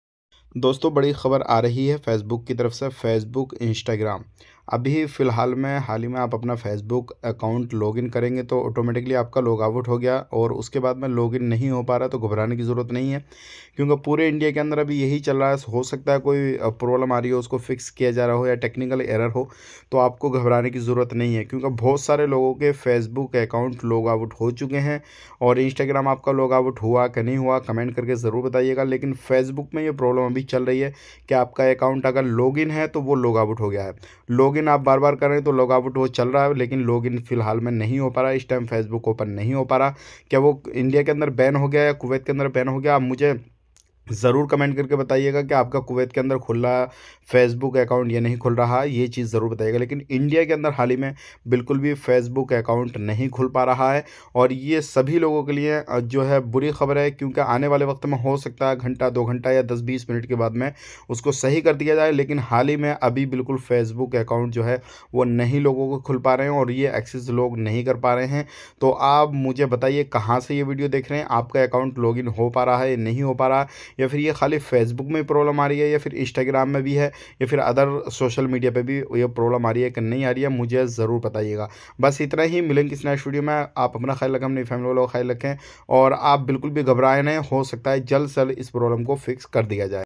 Facebook & Instagram not working server down ! || Facebook Instagram हो रहा है बंद ब्रेकिंग न्यूज
Facebook Instagram ho raha hai band ! || Facebook Instagram हो रहा है बंद ब्रेकिंग न्यूज